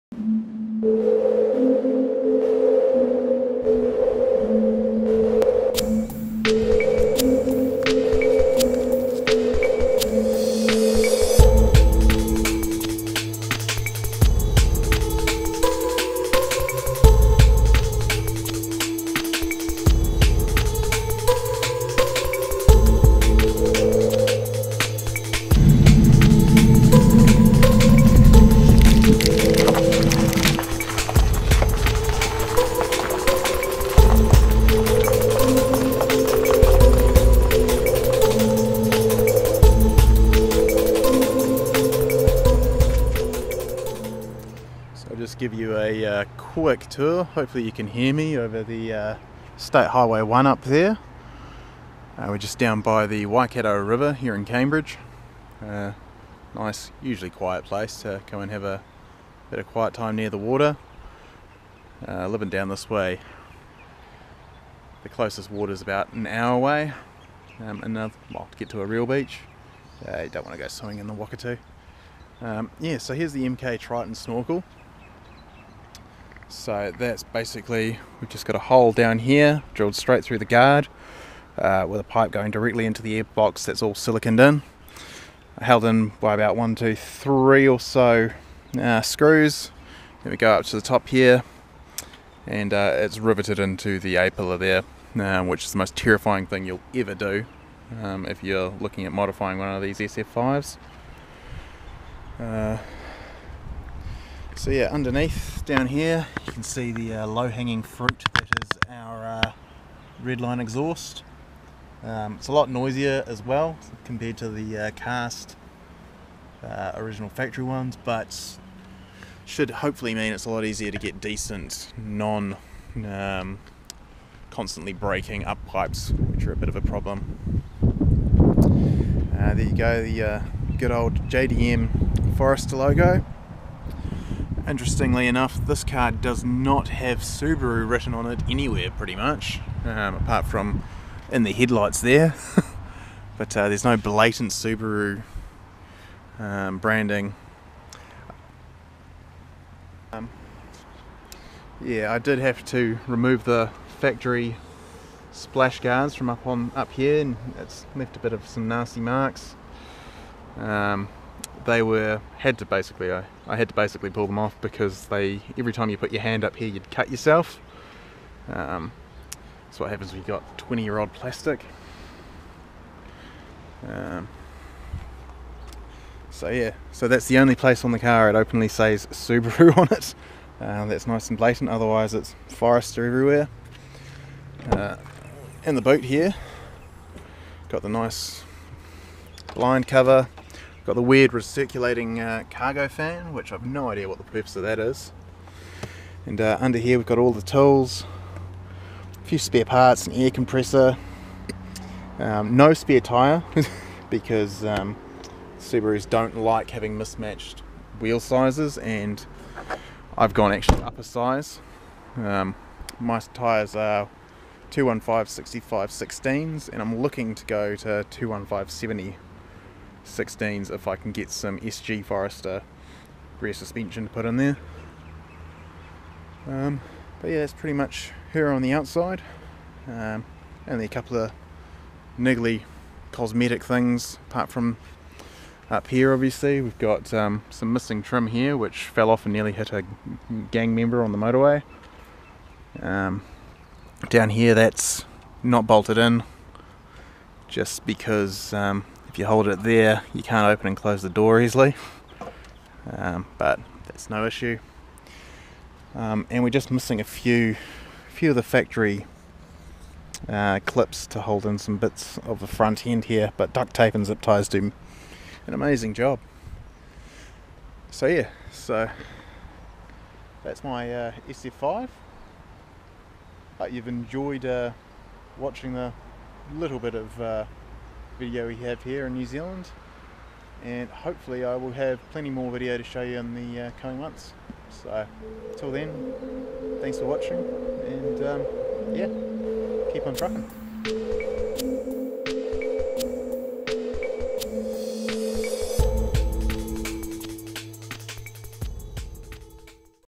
Subaru Forester Overland Project walk around
A quick tour of my 1997 Subaru Forester turbo Overland Project.